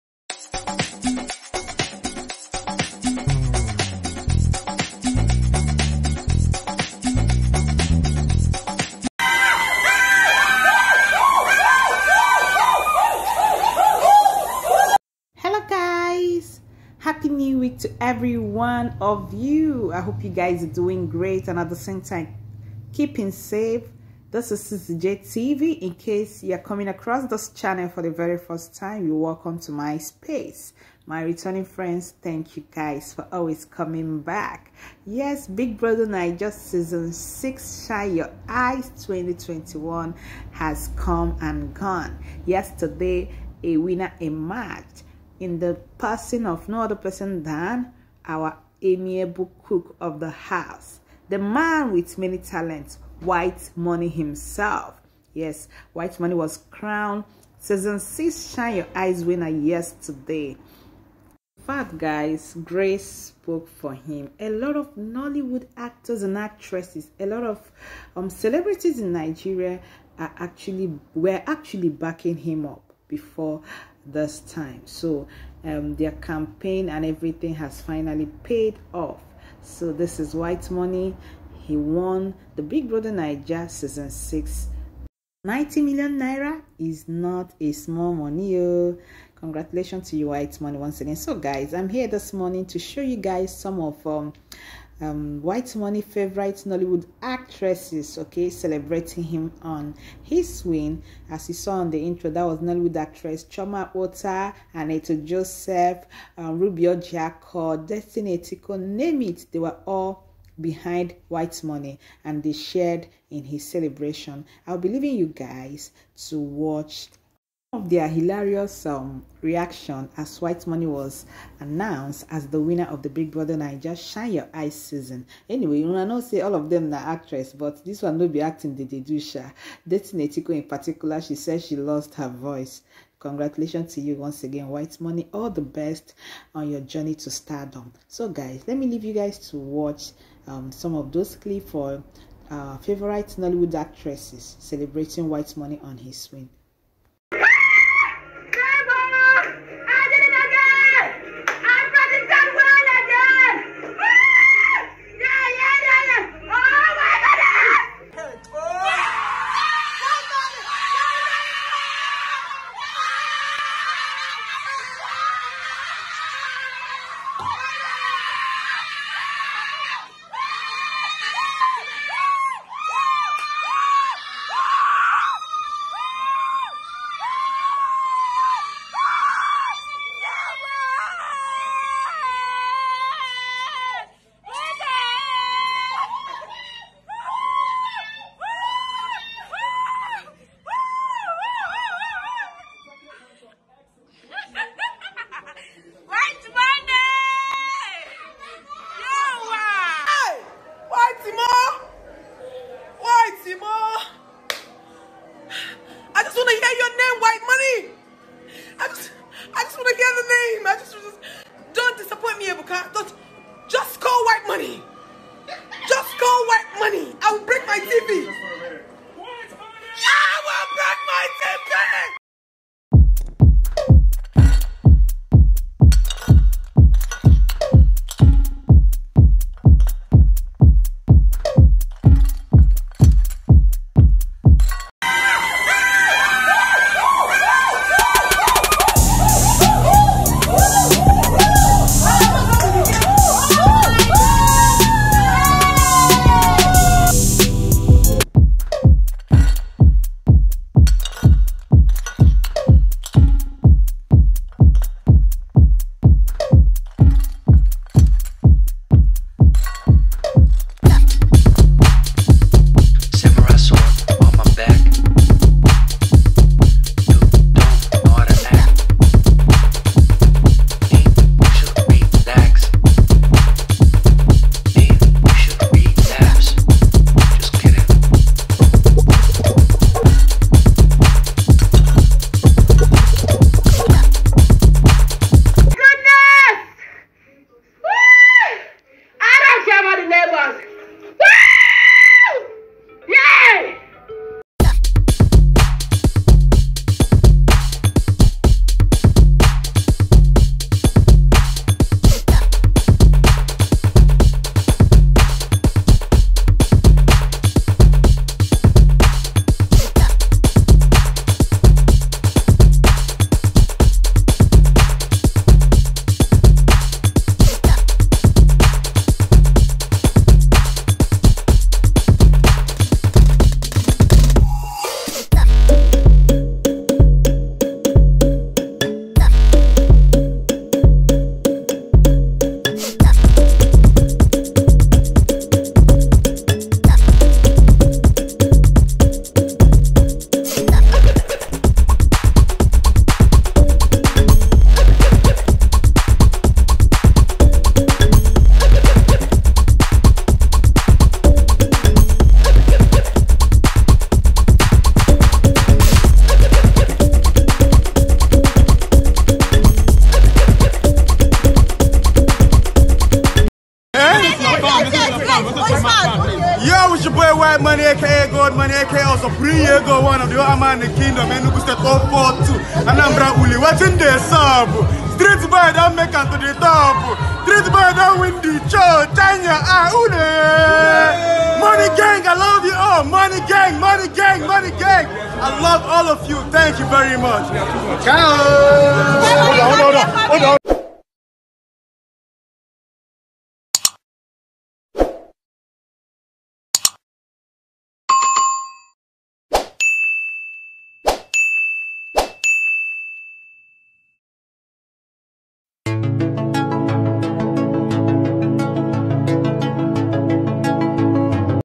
SEE HOW NOLLYWOOD ACTRESSES LOOSE GUARD TO CELEBRATE BBNAIJA WHITEMONEY ON HIS WIN /CHIOMA AKPOTHA
SEE HOW NOLLYWOOD ACTRESSES LOOSE GUARD TO CELEBRATE BBNAIJA  WHITEMONEY ON HIS WIN /CHIOMA AKPOTHA,DESTINY ETIKO,ANITA JOSEPH ,RUBY ORJIAKOR